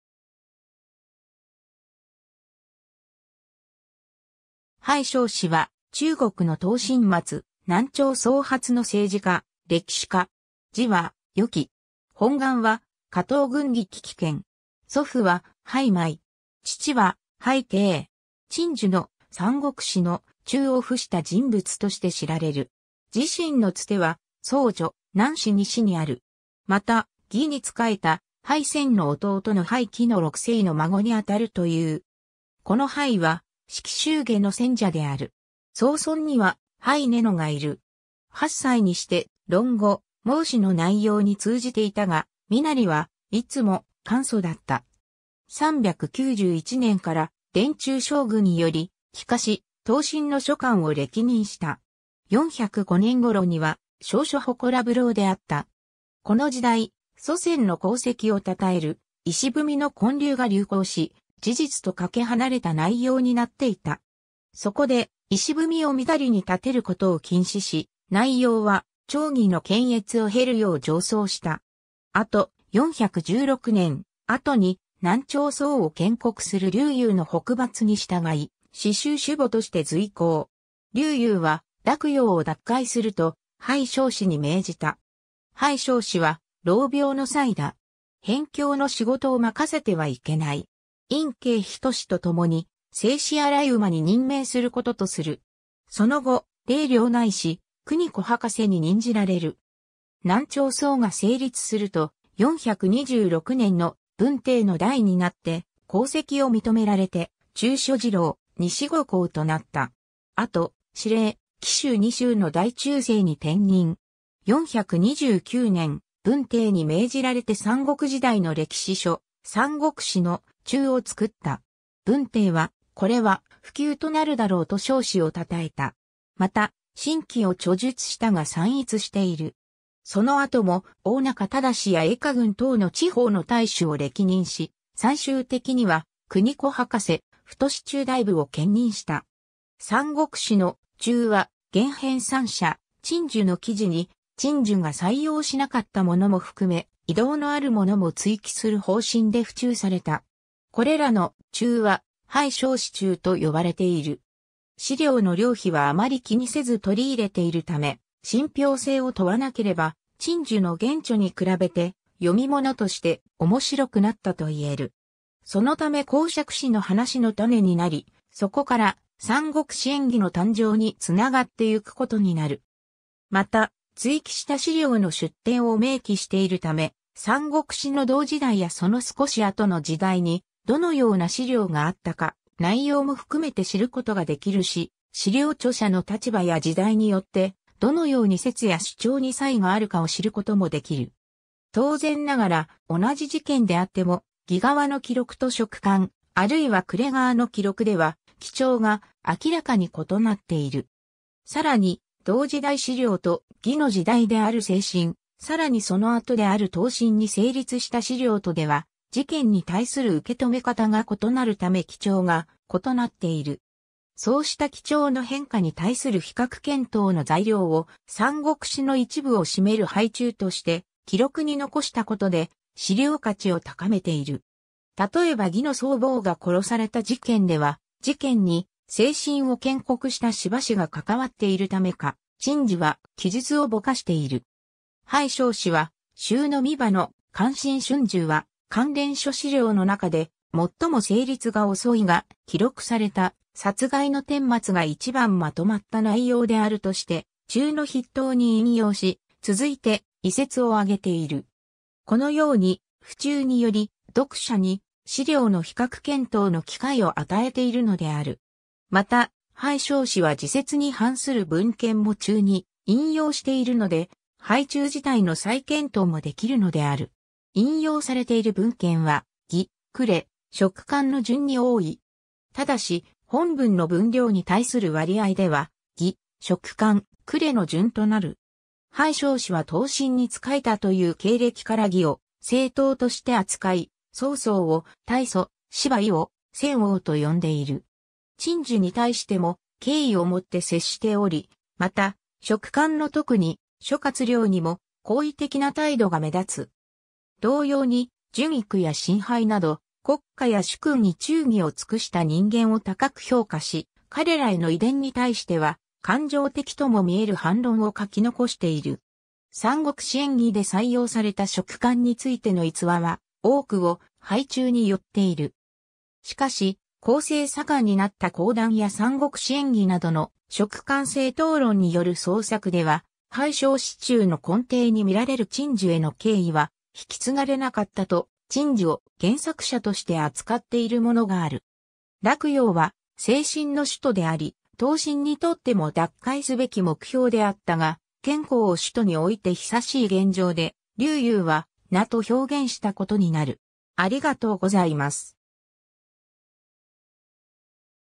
裴松之
裴 松之（はい しょうし、372年 - 451年）は、中国の東晋末・南朝宋初の政治家・歴史家。
字は世期。
本貫は河東郡聞喜県（現在の山西省運城市聞喜県）。
祖父は裴昧。
父は裴珪。
陳寿の『三国志』の「注」を付した人物として知られる。
自身の伝は『宋書』・『南史』二史にある。
また、魏に仕えた裴潜の弟の裴徽の六世の孫にあたるという。
子の裴駰は『史記集解』の撰者である。
曾孫（裴駰の孫）には裴子野がいる。
 8歳にして『論語』『毛詩』の内容に通じていたが、身なりはいつも簡素だった。
 391年（太元16年）から殿中将軍より起家し、東晋の諸官を歴任した。
405年頃には、尚書祠部郎であった。
 この時代、祖先の功績を称える碑の建立が流行し、事実とかけ離れた内容になっていた。
そこで、碑を妄りに建てることを禁止し、内容は朝議の検閲を経るよう上奏した。
後、416年、後に南朝宋を建国する劉裕（武帝）の北伐に従い司州主簿として随行。
劉裕は洛陽を奪回すると、裴松之に命じた。
「裴松之は廊廟の才（国政をになう人材）だ。辺境の仕事を任せてはいけない。殷景仁とともに世子洗馬に任命することとする」。
その後、零陵内史・国子博士に任じられる。
 南朝宋が成立すると、426年（元嘉3年）の文帝の代になって、功績を認められて、中書侍郎・西郷侯となった。
後、司隷・冀州二州の大中正に転任。
429年（元嘉6年）、文帝に命じられて三国時代の歴史書『三国志』の「注」（裴注）を作った。
文帝は「これは不朽となるだろう」と松之をたたえた。
また、『晋記』を著述したが散逸している。
 その後も大中正や永嘉郡等の地方の太守を歴任し、最終的には国子博士・太中大夫を兼任した。
 『三国志』の「注」は原編纂者陳寿の記事に、陳寿が採用しなかったものも含め、異同のあるものも追記する方針で付注された。
これらの「注」は「裴松之注」（略して「裴注」）と呼ばれている。
 史料の良否はあまり気にせず取り入れている（「信用できない史料である」などとことわったりしながらも載せている）ため、信憑性を問わなければ、陳寿の原著に比べて読み物として面白くなったと言える。
そのため講釈師の話の種になり、そこから『三国志演義』の誕生につながってゆくことになる。
 また、追記した史料の出典を明記しているため、三国志の同時代やその少し後の時代にどのような史料があったか、内容も含めて知ることができるし、史料著者の立場や時代によって、どのように説や主張に差異があるかを知ることもできる。
当然ながら、同じ事件であっても魏側の記録と蜀漢、あるいは呉側の記録では基調が明らかに異なっている。
さらに、同時代史料と、魏の次代である西晋、さらにその後である東晋に成立した史料とでは、事件に対する受け止め方が異なるため基調が異なっている。
そうした基調の変化に対する比較検討の材料を、三国志の一部を占める裴注として記録に残したことで史料価値を高めている。
 例えば魏の曹髦が殺された事件では、事件に西晋を建国した司馬氏が関わっているためか、陳寿は記述をぼかしている。
裴松之は習鑿歯の『漢晋春秋』は関連諸資料の中でもっとも成立が遅いが、記録された殺害の顛末が一番まとまった内容であるとして、注の筆頭に引用し、続いて異説を挙げている。
このように、付注により読者に史料の比較検討の機会を与えているのである。
また、裴松之は自説に反する文献も注に引用しているので、裴注自体の再検討もできるのである。
 引用されている文献は、魏・呉・蜀漢の順に多い。
ただし、本文の分量に対する割合では、魏・蜀漢・呉の順となる。
裴松之は東晋に仕えたという経歴から魏を正統として扱い、曹操を太祖、司馬懿を宣王と呼んでいる。
陳寿に対しても敬意を以て接しており、また蜀漢の特に諸葛亮にも好意的な態度が目立つ。
同様に荀彧や審配など国家や主君に忠義を尽くした人間を高く評価し、彼らへの異伝に対しては感情的とも見える反論を書き残している。
『三国志演義』で採用された蜀漢についての逸話は、多くを裴注に拠っている。
しかし、後世盛んになった講談や三国志演義などの蜀漢正統論による創作では、裴松之注の根底に見られる陳寿への敬意は引き継がれなかった（ただし、『三国志演義』は刊本によっては「晋平陽侯陳壽史傳。後學羅本貫中編次」（明の嘉靖年間の版本）と、陳寿を原作者として扱っているものがある）。...